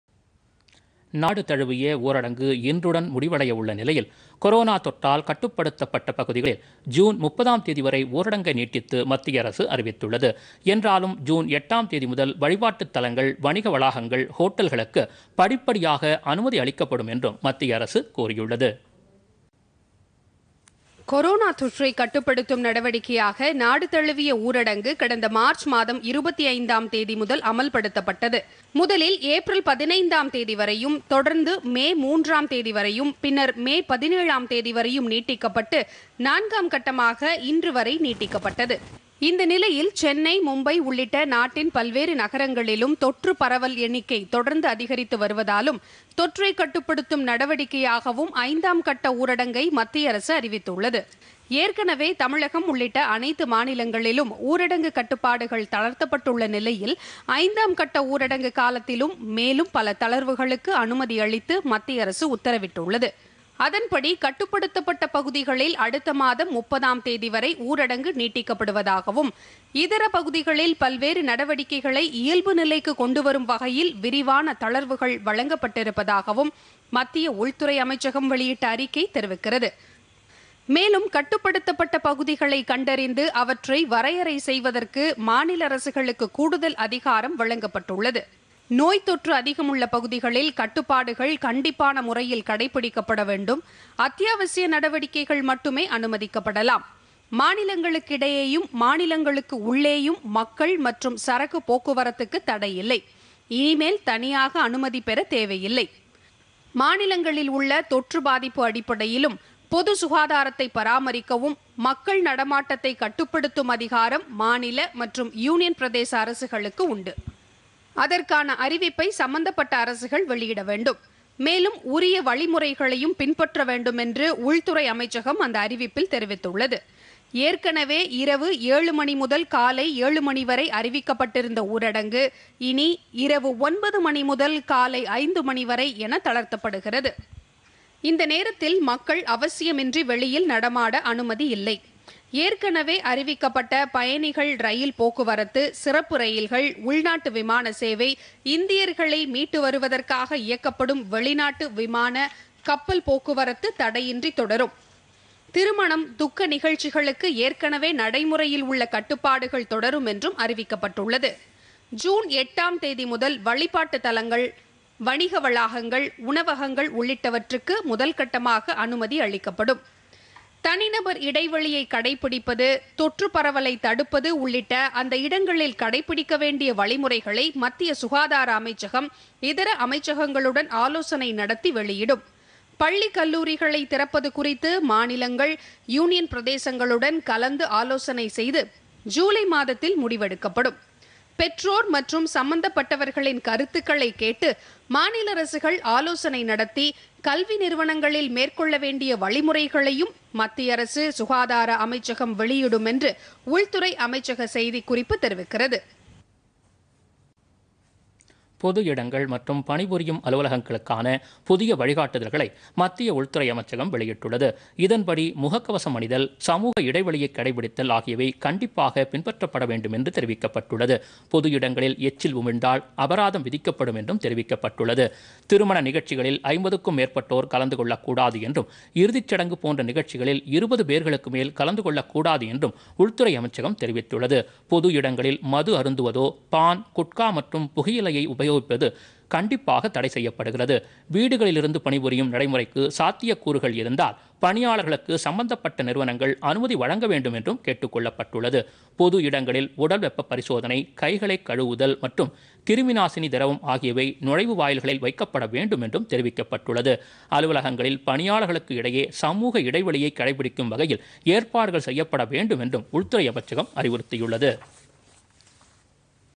ஜூன் 30-ஆம் தேதிவரை ஊரடங்கு நீட்டிப்புPodhigaiTamilNews
நாடு முழுவதும் கட்டுப்படுத்தப்பட்ட பகுதிகளில் பொது ஊரடங்கு ஜூன் முப்பதாம் தேதிவரை நீட்டிப்பு: பயணிகள் ரயில்கள், உள்நாட்டு விமான சேவையை கட்டுப்பாடுகளுடன் தொடர மத்திய அரசு அனுமதி.